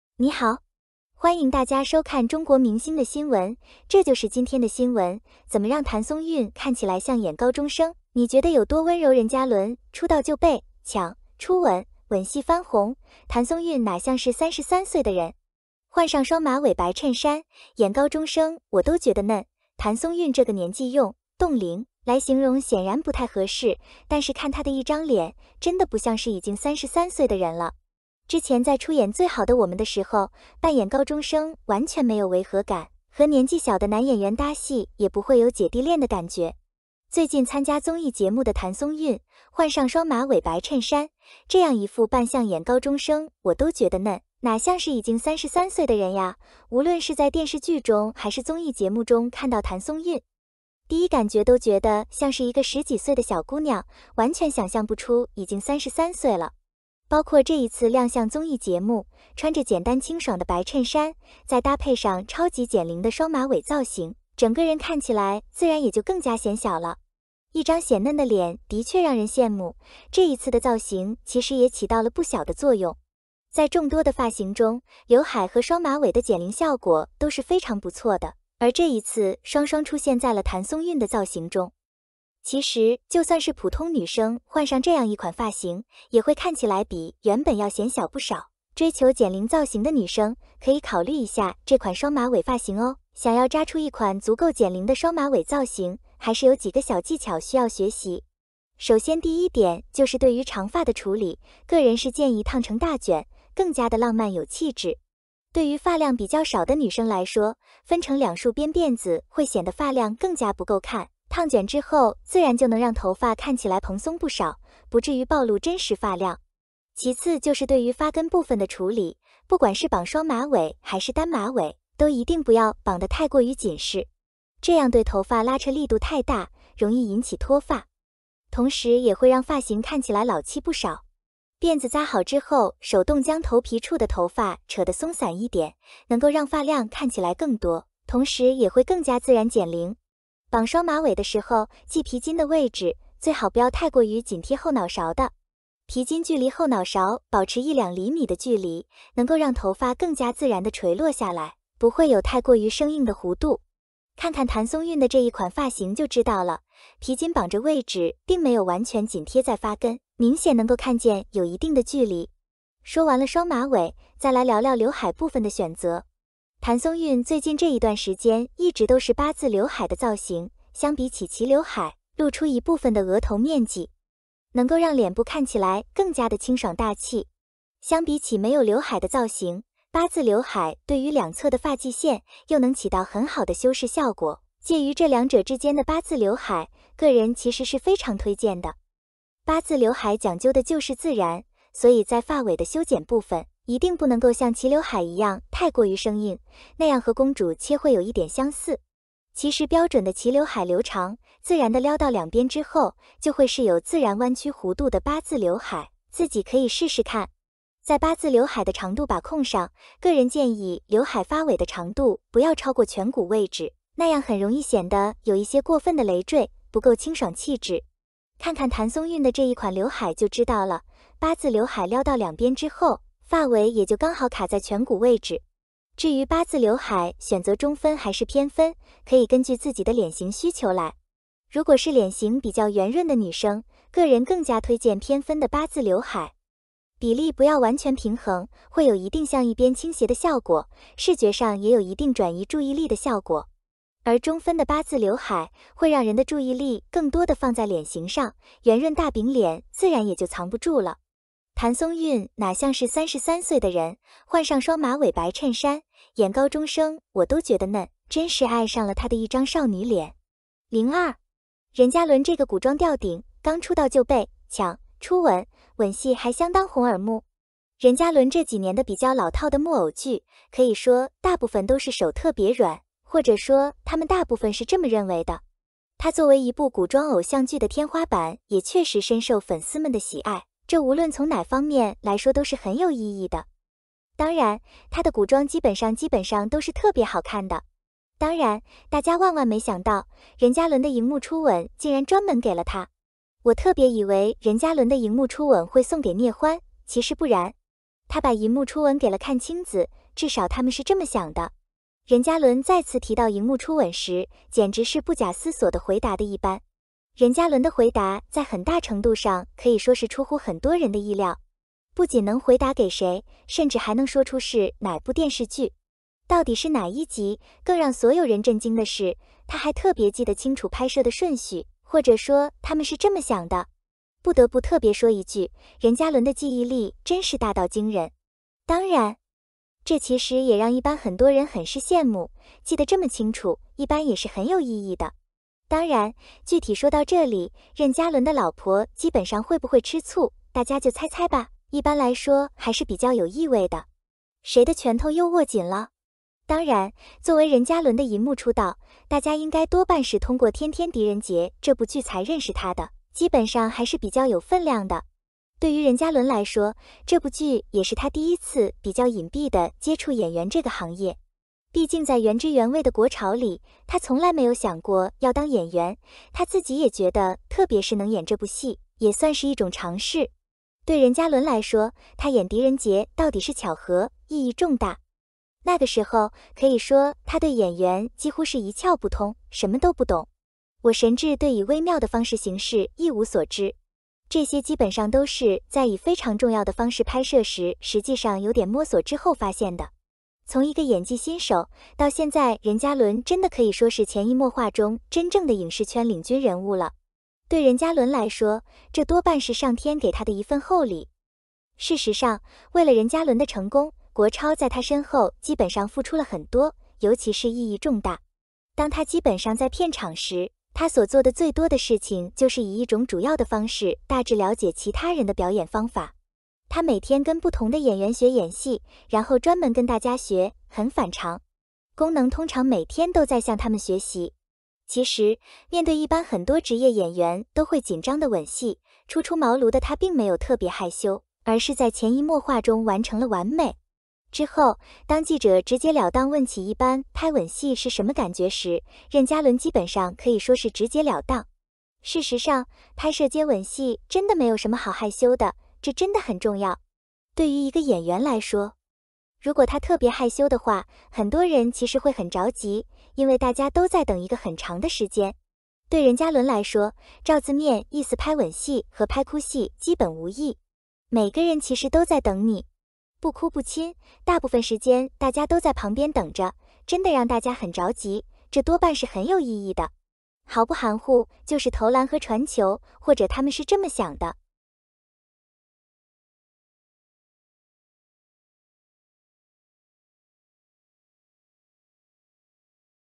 怎么让谭松韵看起来像演高中生，你觉得有多温柔任嘉伦，出道就被“抢”初吻，吻戏翻红？
古偶天花板任嘉倫，剛出道就被「奪」走初吻，拍吻戲紅透耳朵根？
谭松韵哪像是30岁的人？换上双马尾白衬衫，演高中生我都觉得嫩